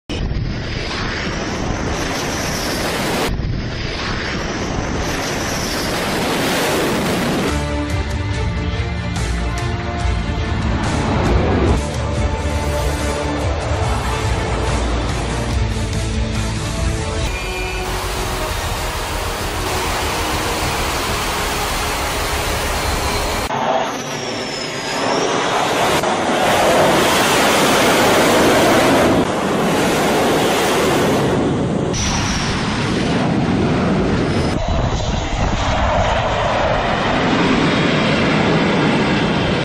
함재기 착함 비교 - 중국,프랑스,러시아,미국,인도
1.중국 - J15 산둥함 함재기 착함
2.프랑스 - 닷쏘社 라팔(Lerafale F-3), 미라쥬 F-1 함재형
3.러시아 - 중항공순양함에 Su-33 어레스팅 후크 착함
4.미국 - F18 슈퍼호넷, EA-6B 프라울러 전자전기 항공모함 착륙
5.인도 - 비크란트 항모에 착함하는 테자스 함재기